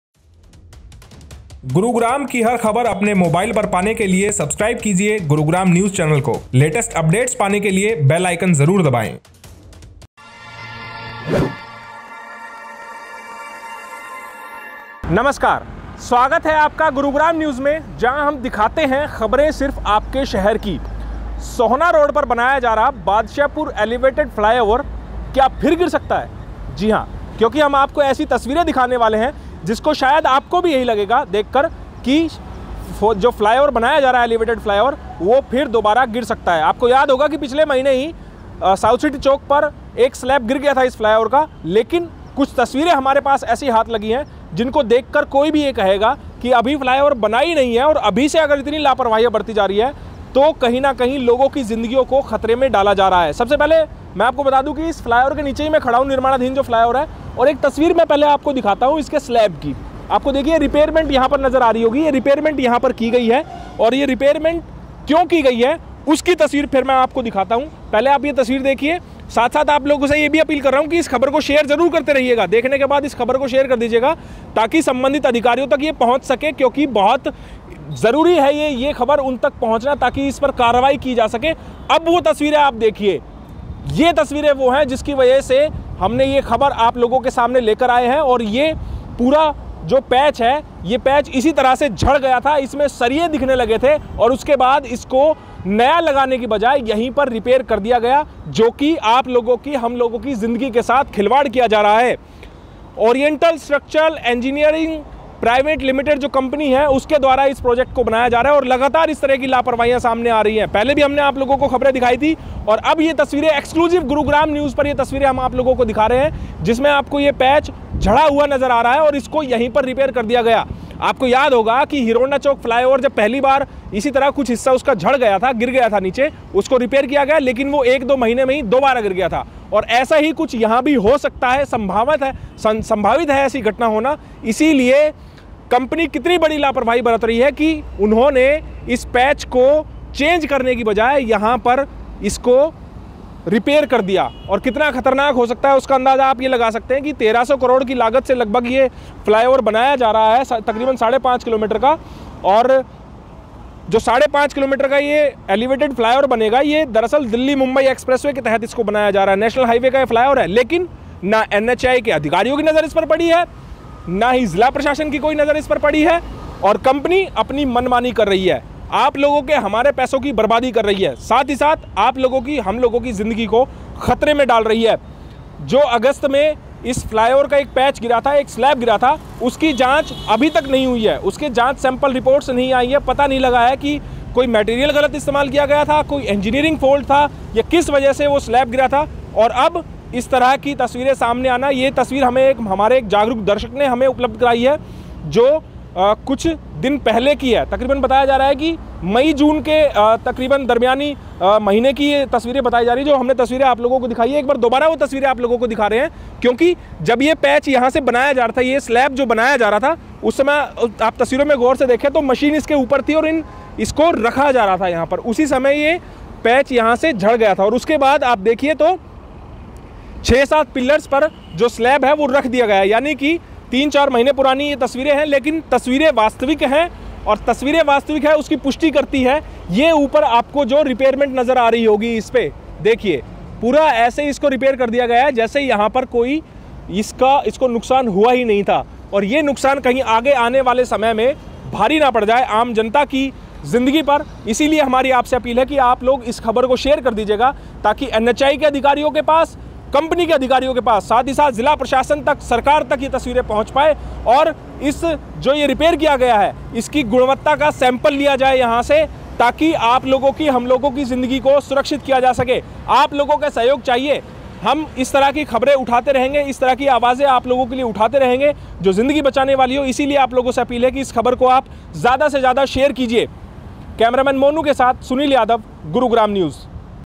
फिर गिर सकता है Sohna Road का एलिवेटेड फ्लाइओवर - GurugramNews
पिछले महीने सोहना रोड़ बनाए जा रहे एलिवेटेड फ्लाइओवर का स्लैब गिर गया जिसके बाद अब ऐसी तस्वीर सामने आई है जिन्हें देखकर लगता है कि इस फ्लाइओवर को बनाने में बहुत सी लापरवाहियां बरती जा रही है कहीं ऐसा ना हो कि इन लापरवाहियों के चलते ये फ्लाइओवर फिर से गिर जाए । देखिए हमारी Exclusive Report